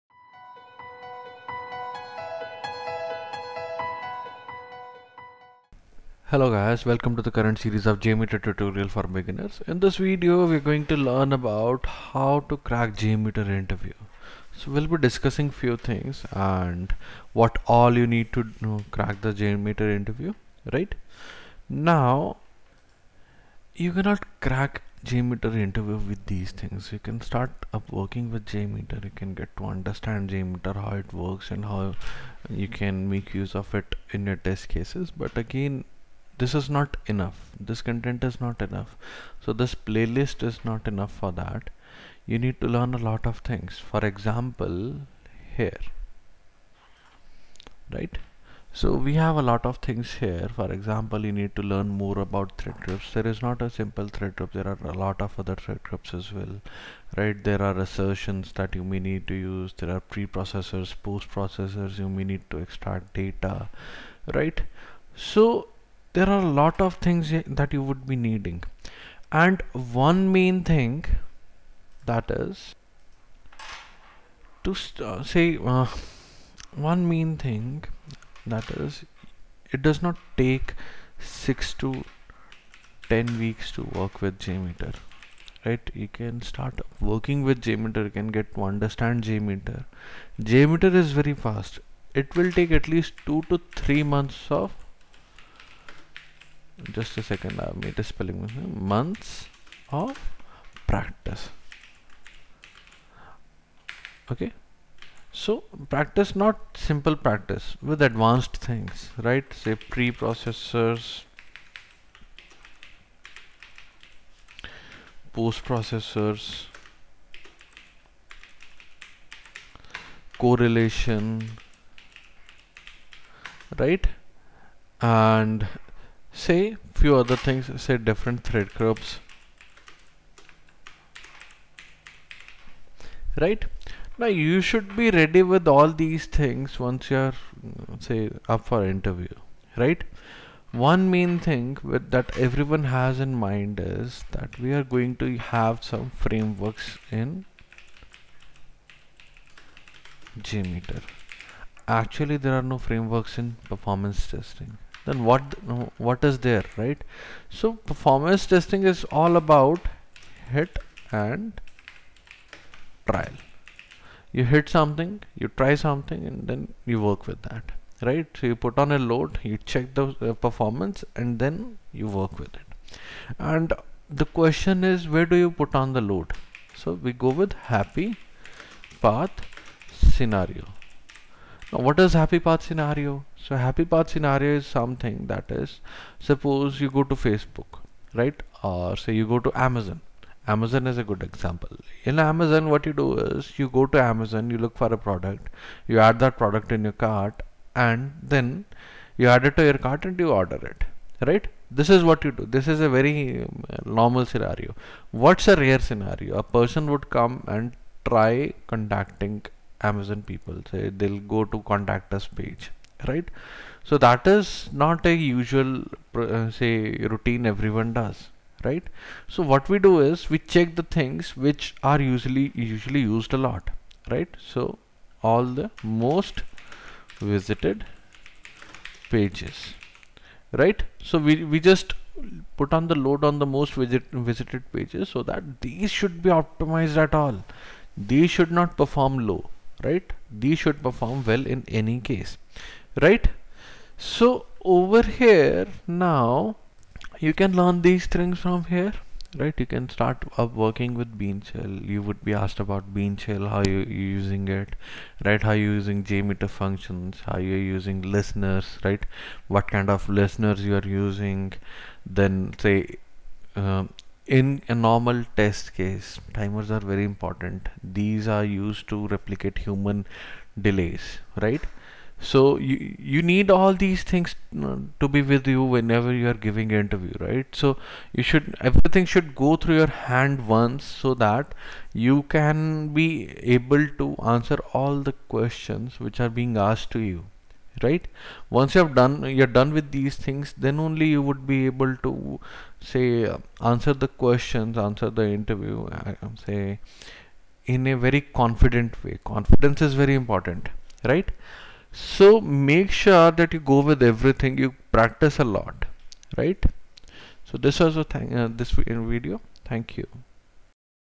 How To Crack JMeter Interview?
* Introduction and Basics

- Introduction to Performance Testing
- Automated Script Generation
- Manual Script Generation

* Script Building Essentials

- Logic Controllers
- Config Elements
- Thread Groups
- Assertions
- Pre-Processors
- Post-Processors

* Timers, Listeners and Functions

- Timers
- Listeners
- JMeter Functions

* BeanShell Scripting

* Correlation

* Plugins, Files, Databases and WebServices

- Plugins in JMeter
- File Download And Upload
- Testing Databases with JMeter
- Testing WebServices in JMeter

* Reporting and Command-line

- Non-GUI (Command Line)
- JMeter in Distributed Mode
- Report Analysis And Monitoring Resources
- Grafana

* JMeter Selenium Integration